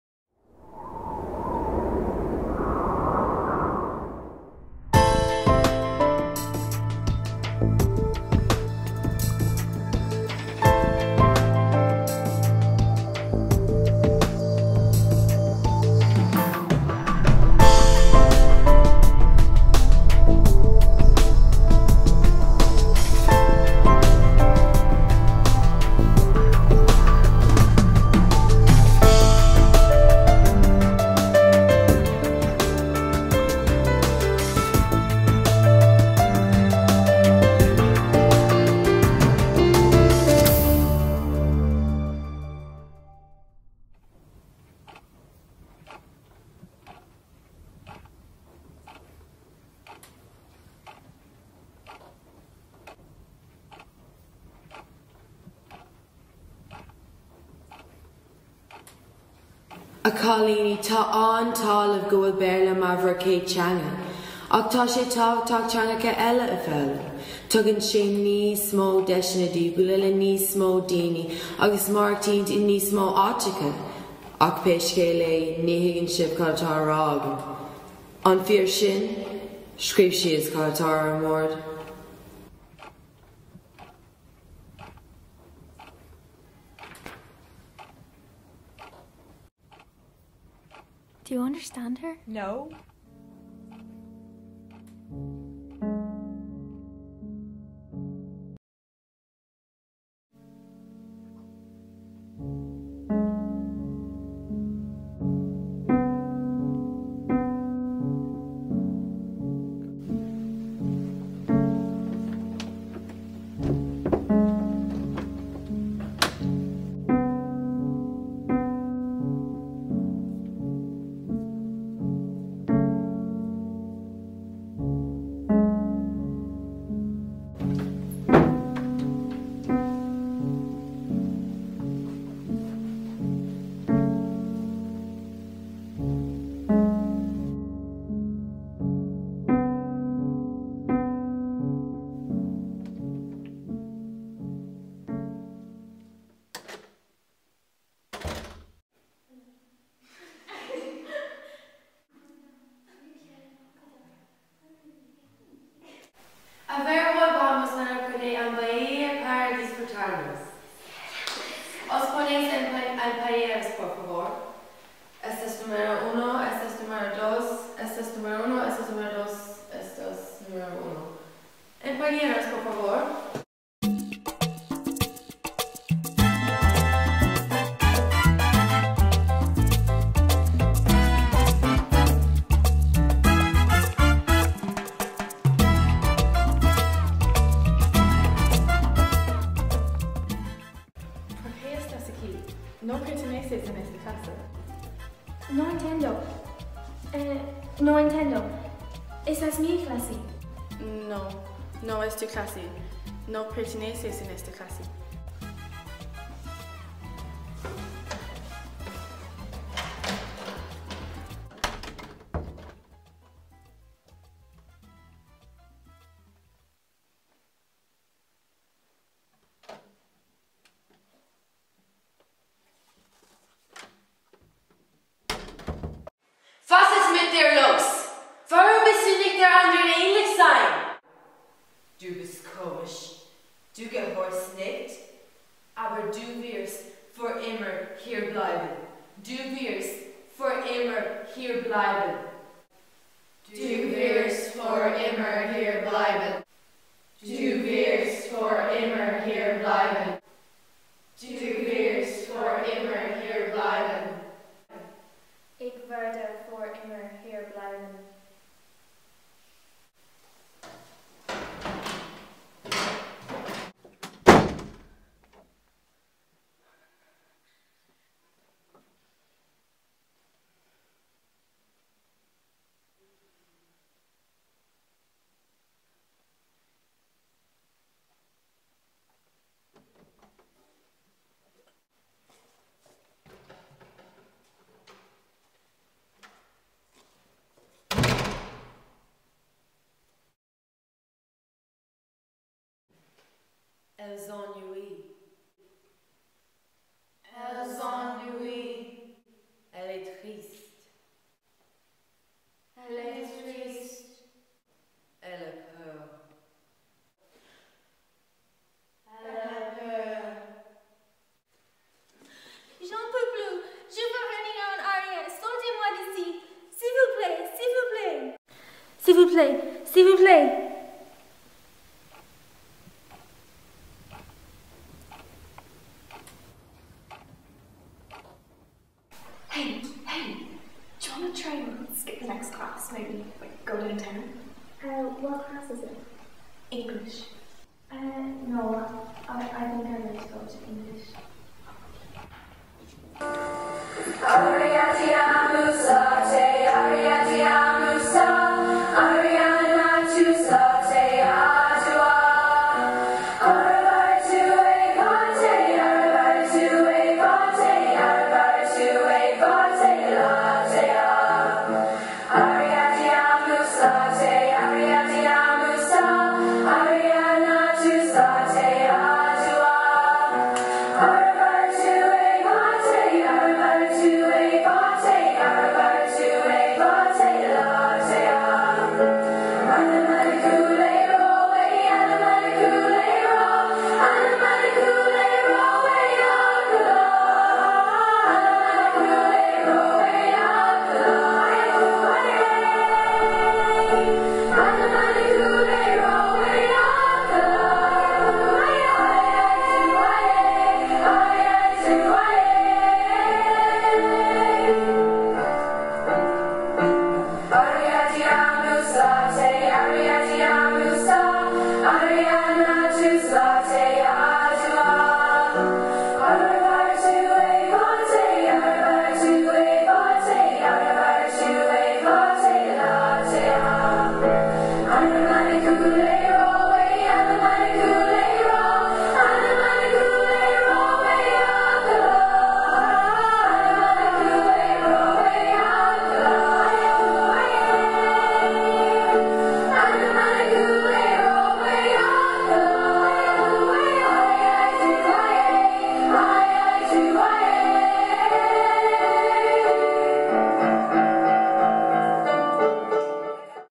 Typing Room
Typing Room is the result of a film project undertaken by TY students and a few teachers in Presentation Wexford in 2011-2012. The film was made using iMovie. The students wrote the script, did the storyboard, acted, filmed, edited. The film unfortunately was not eligible for the FIS competition as we are a secondary school. However, the Wexford Education Centre allowed to have it shown on the big screen with the other "primary schools"' entries.